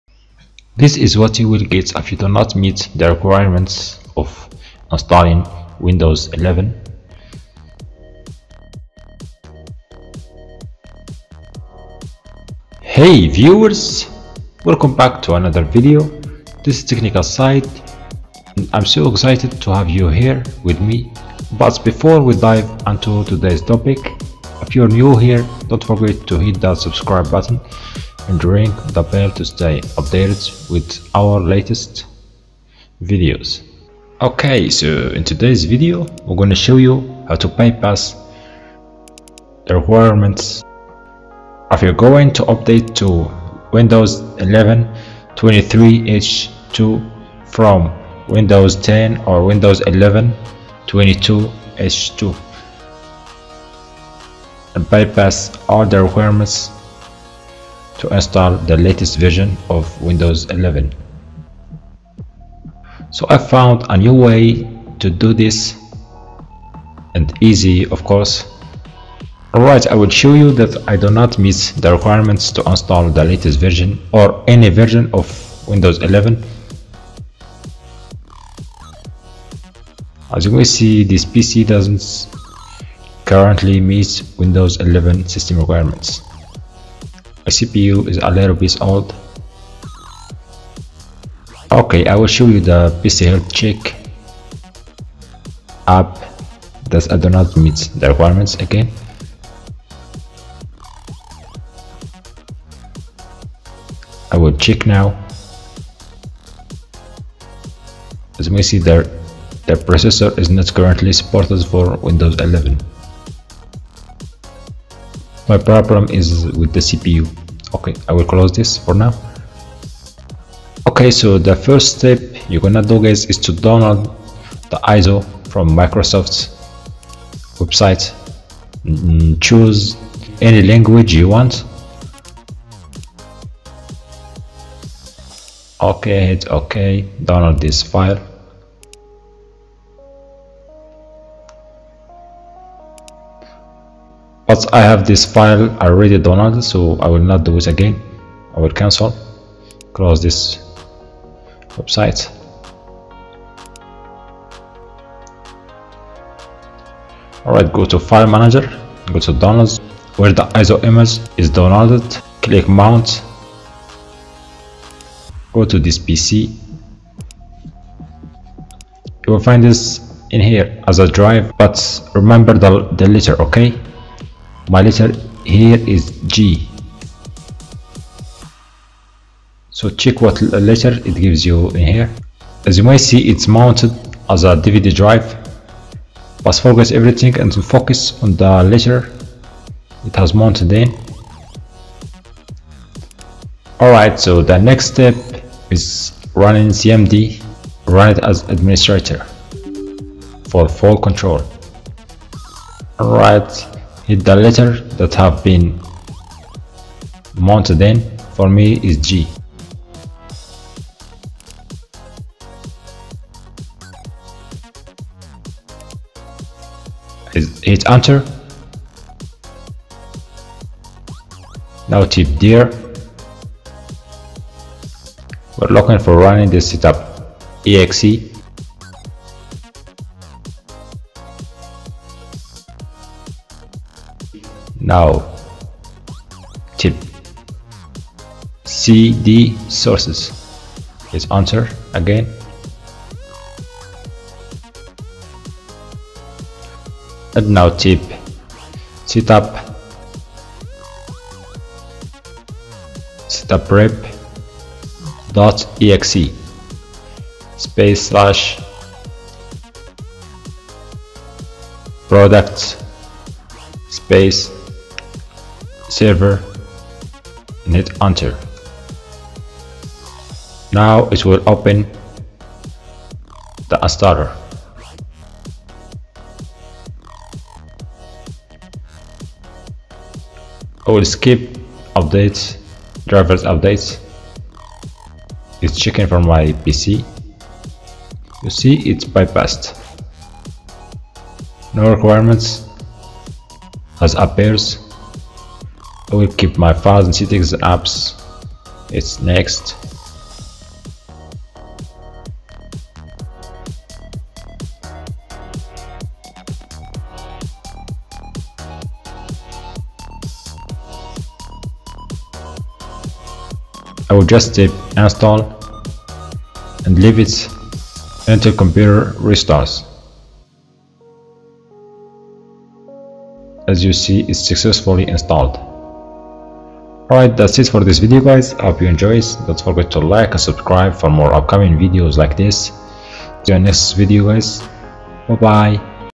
Update to Windows 11 23H2 and Bypass TPM, CPU and RAM Requirements on UNSUPPORTED HARDWARE
Microsoft has some strict hardware requirements that your PC must meet to install Windows 11, including TPM 2.0 support. This means that not only older computers, but virtual machines will refuse to upgrade from Windows 10, giving you a message that "this PC doesn't currently meet Windows 11 system requirements."

Commands
dir
cd sources
setupprep.exe /product server